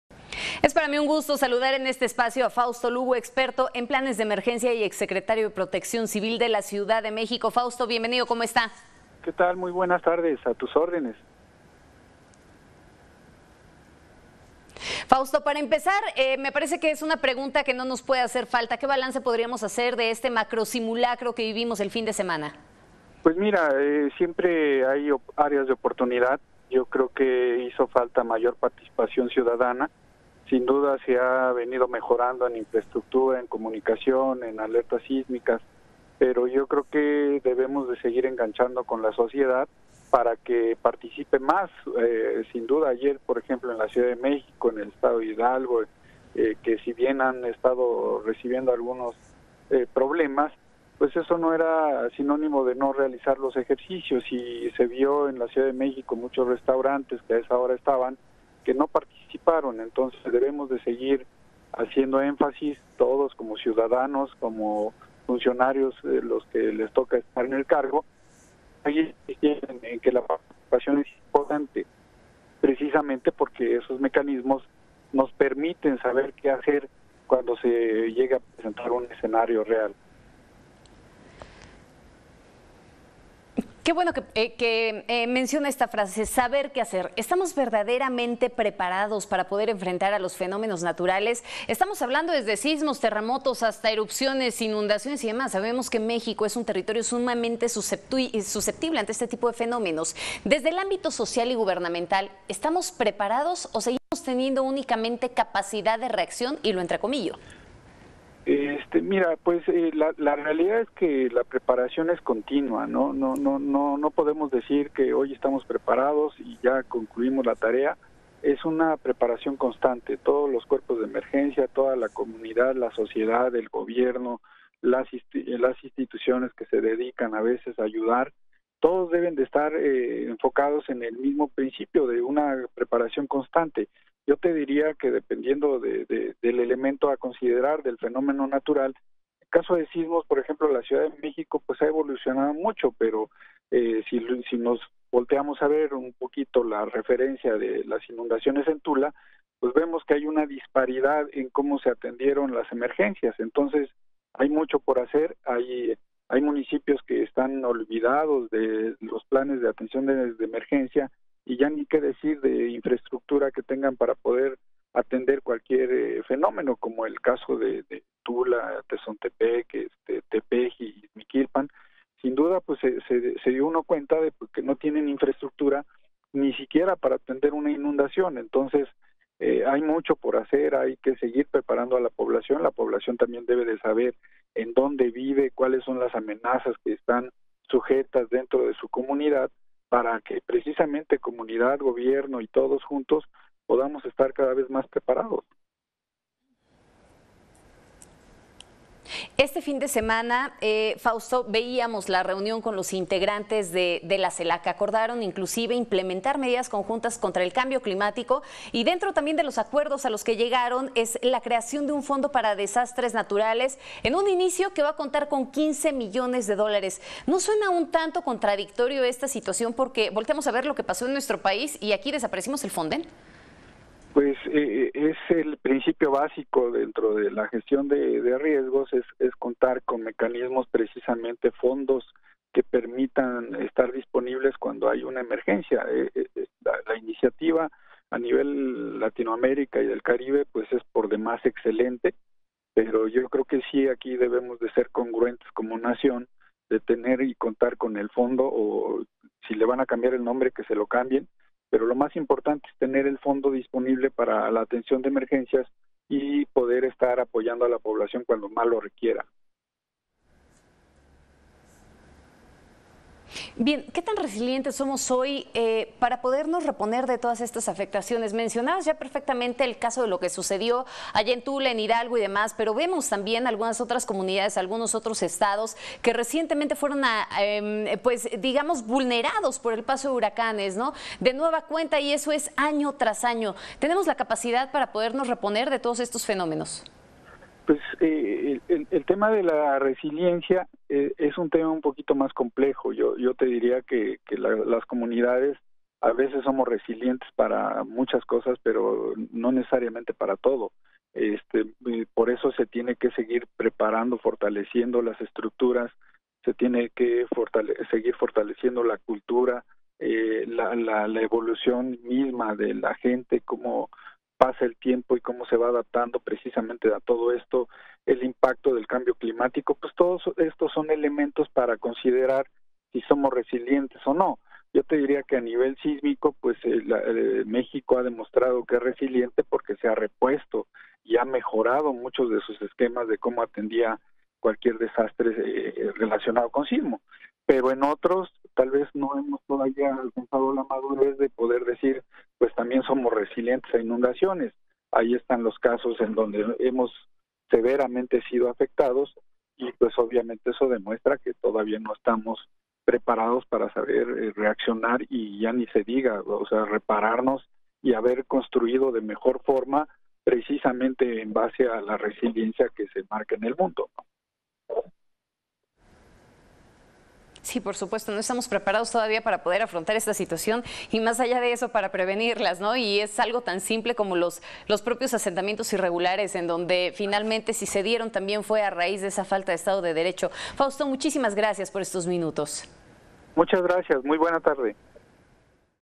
¿Estamos realmente preparados para poder enfrentar a los fenómenos naturales?
Magda Bastida en entrevista con Fausto Lugo, Experto en planes de emergencia y ex secretario de protección civil de la ciudad de México